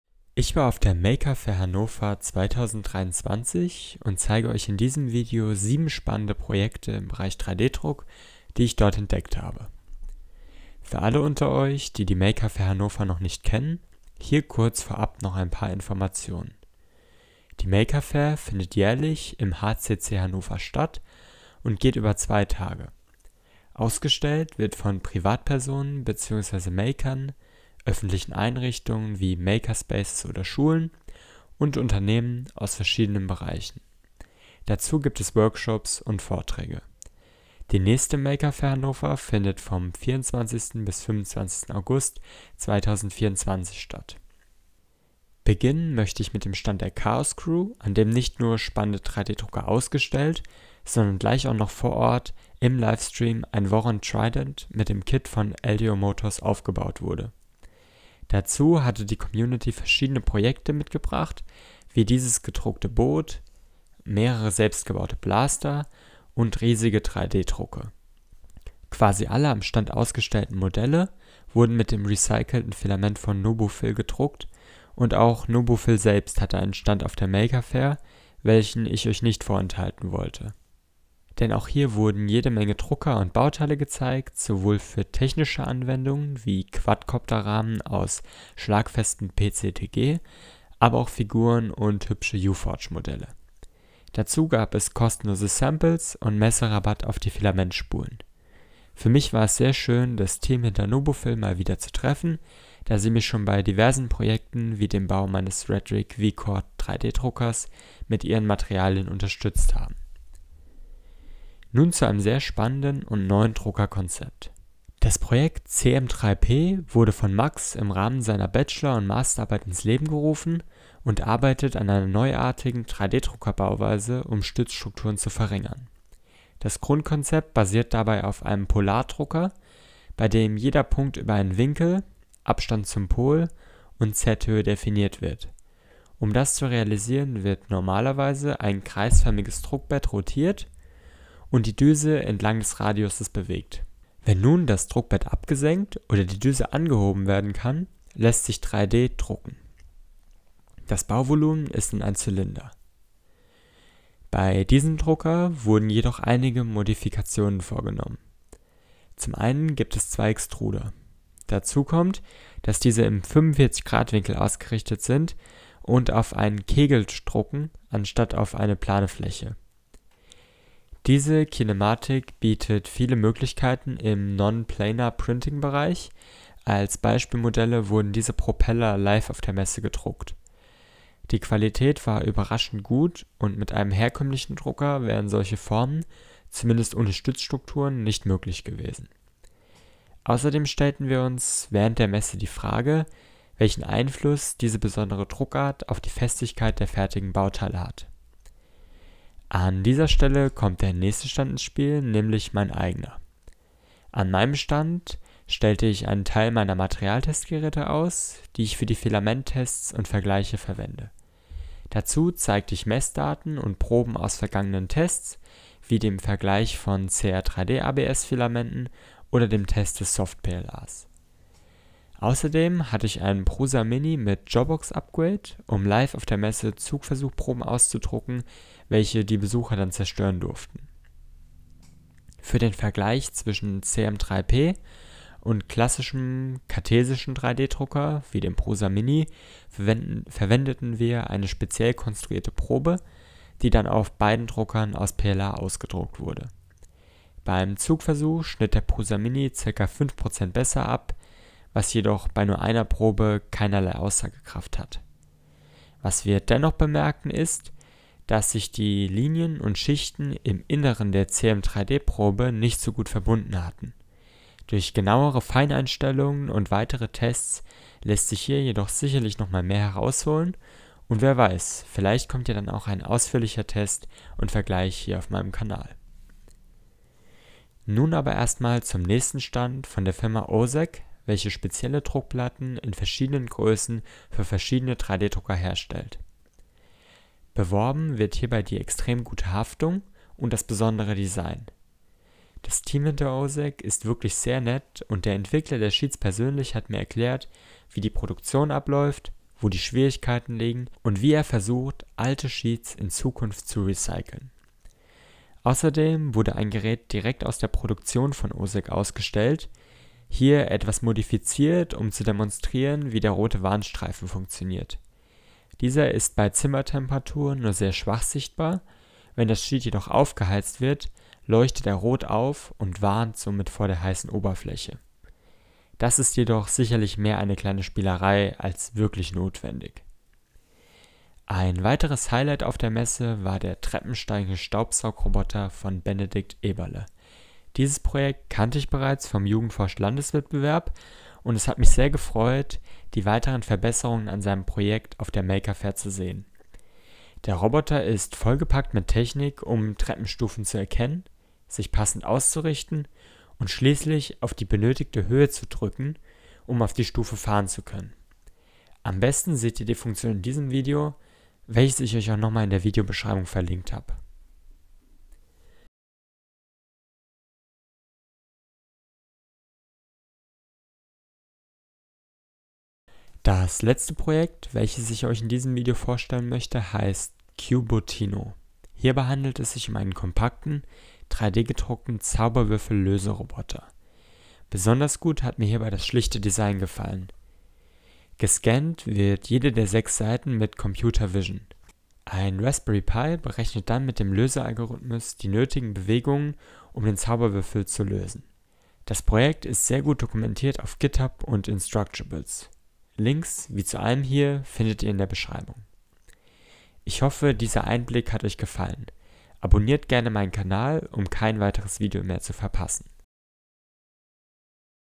7 spannende 3D-Druck Projekte auf der Maker Faire Hannover 2023!
Ich war als Aussteller auf der Maker Faire Hannover dabei und zeige Euch in diesem Video einige spannende Projekte rund um das Thema 3D-Druck, die ich dort gefunden habe.

Kapitel:
00:00 Einleitung
00:45 Chaoscrew
01:15 Nobufil
01:51 CM3P Non-Planar-Drucker
03:19 Materialprüfung bei JanTec (Mein Stand)
04:37 OSEQ
05:31 TSR
06:13 CUBOTino

JanTec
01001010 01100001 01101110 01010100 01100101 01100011